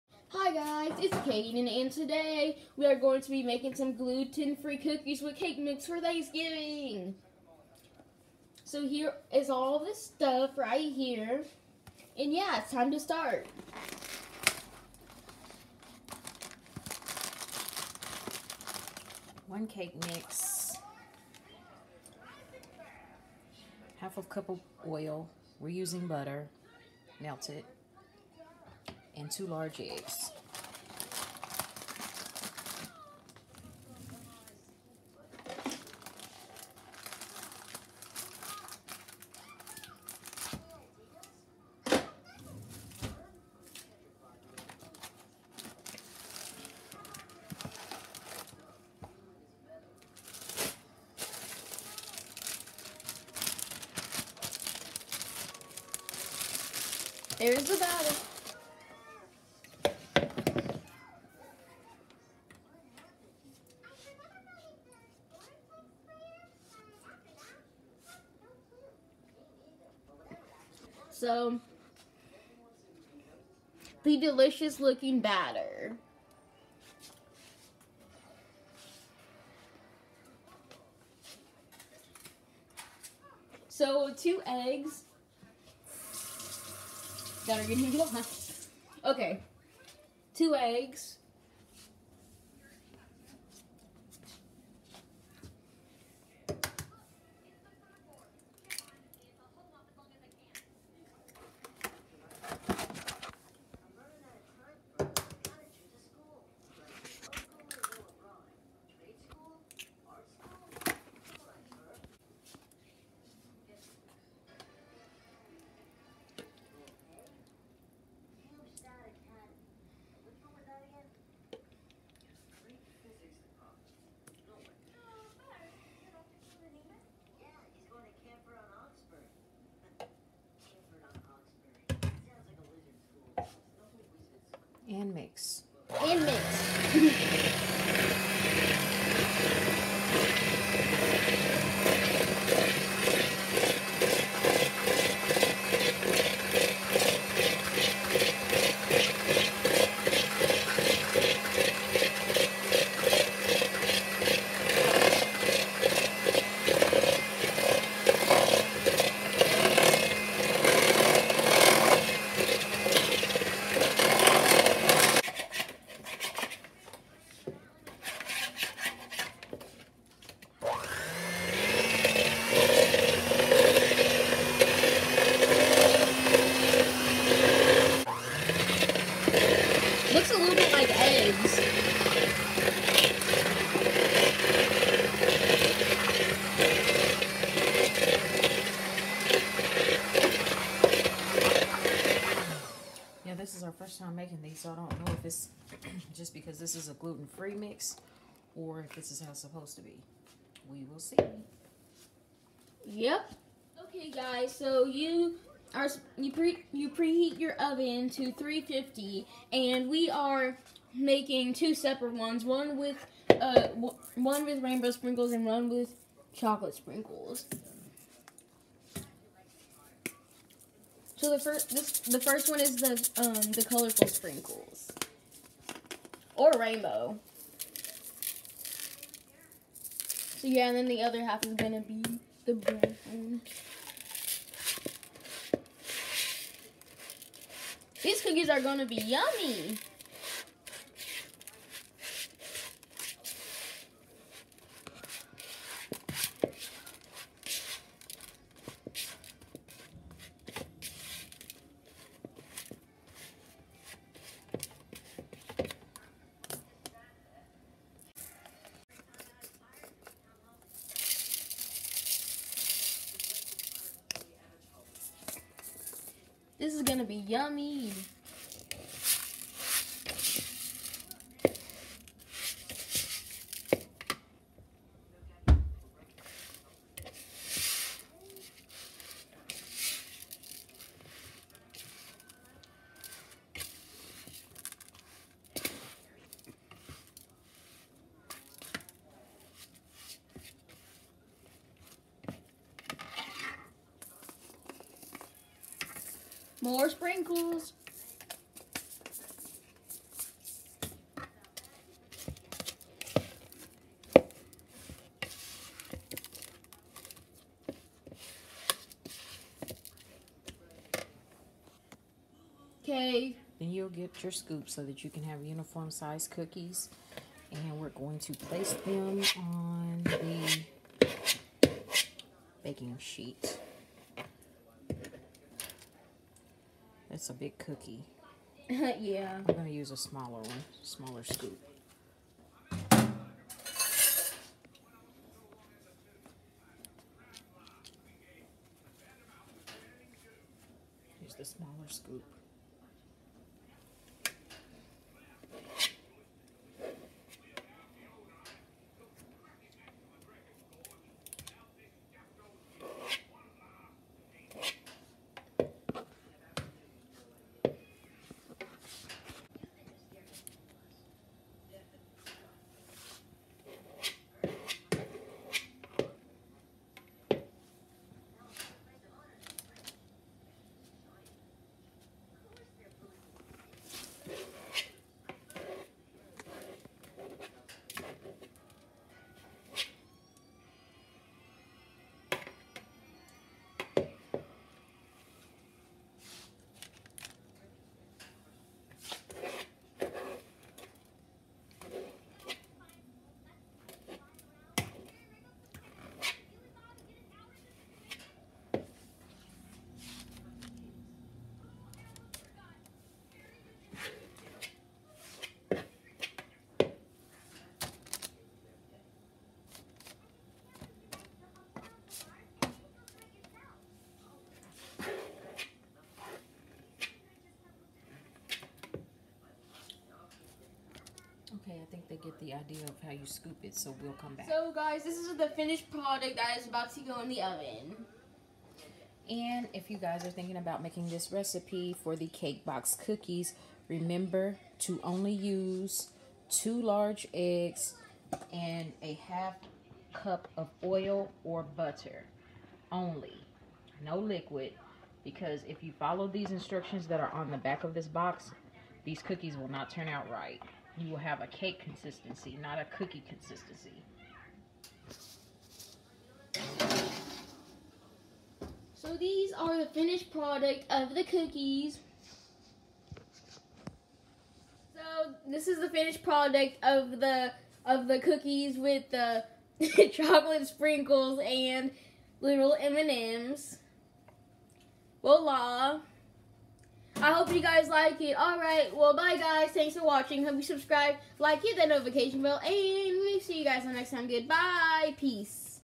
making gluten free cookies for thanksgiving!/recipe
we’re making cookies! gluten free! Hope y’all have a good thanksgiving! 🥺💗